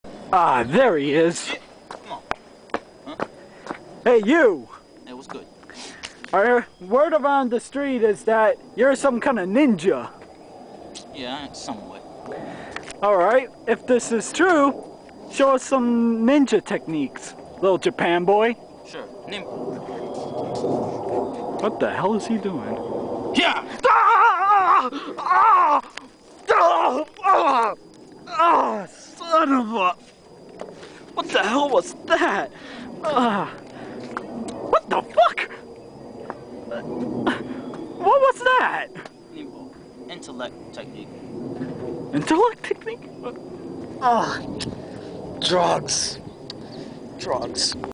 Little japan boy
yea were bored and made this based on naruto

yea sorry i sound off because i keep on forgeting my lines lol and yea the ending is kinda dumb but o well..